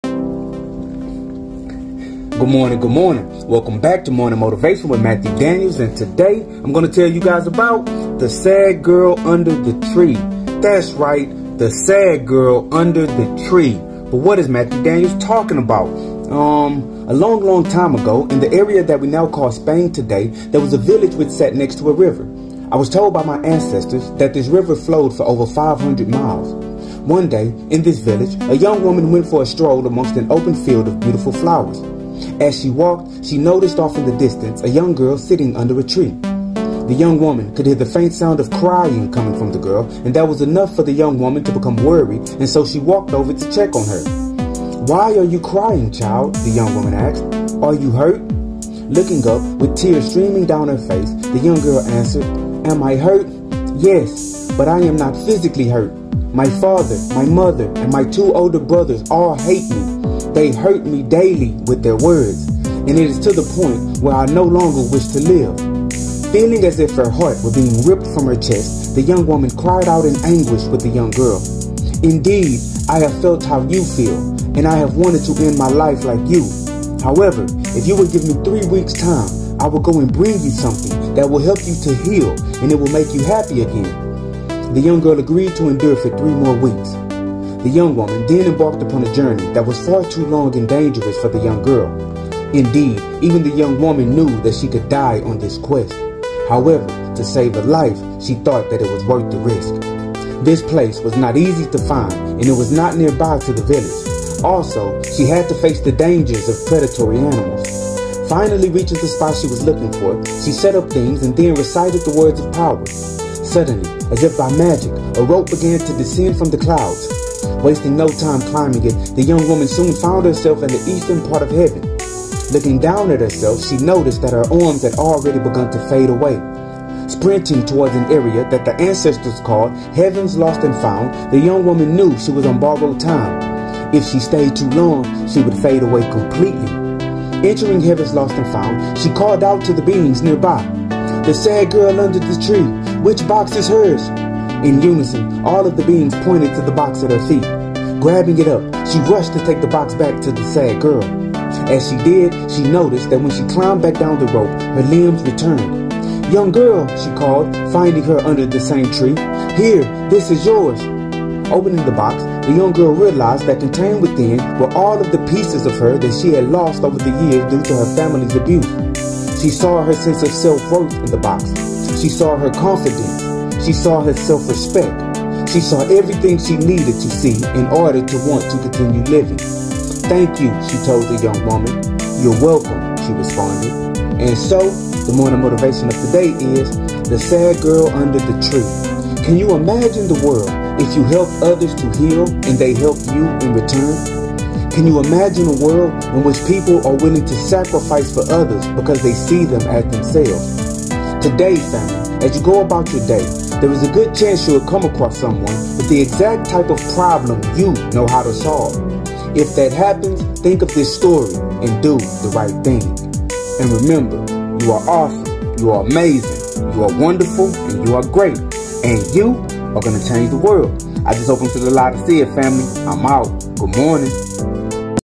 The Sad Girl Under the Tree: Morning Motivation with Matthew Daniels
I am the author of five books. All books by The Scribe Matthew Daniels available at DandyandBigHerm.com and Amazon.